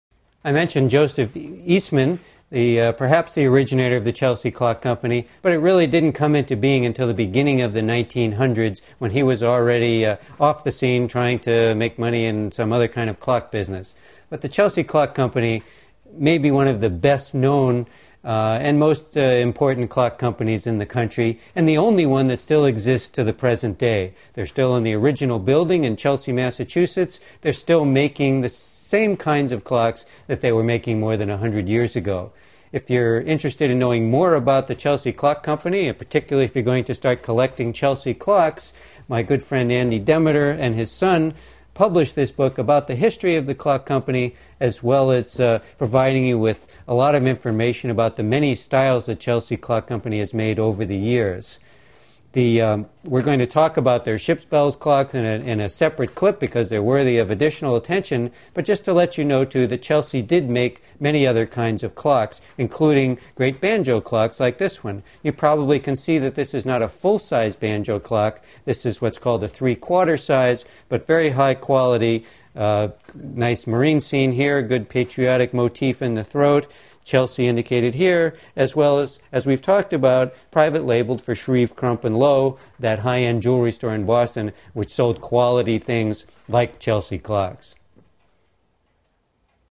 Antique Clock Collecting: Chelsea Clock Company
Antique Clock Collecting: Chelsea Clock Company. Part of the series: Antique Clock Collecting: Boston Styles. The Chelsea Clock company, producer of many sought-after antique clocks, was founded by Joseph Eastman. Discover why antique collectors value Chelsea clocks in this free video on collecting antique clocks from Boston from an expert in clock restoration.